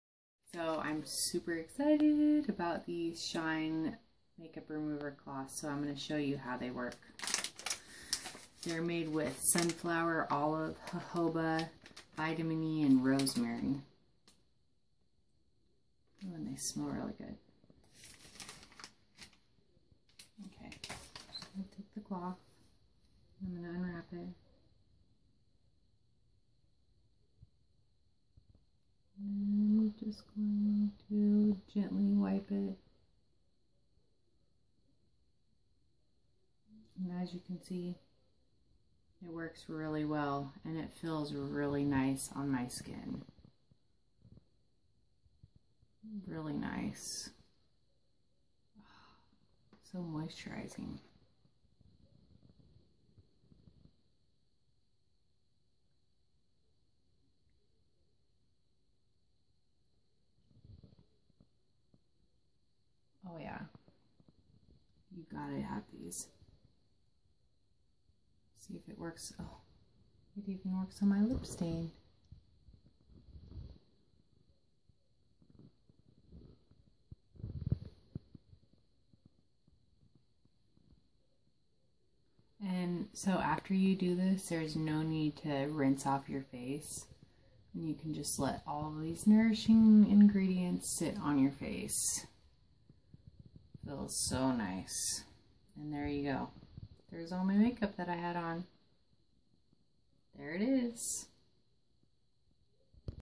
New Shine Makeup removing cloths
My face feels so nice after using these cloths and does an excellent job taking off my makeup, even my 3D mascara and Precision Pencil liner! 💜💜💜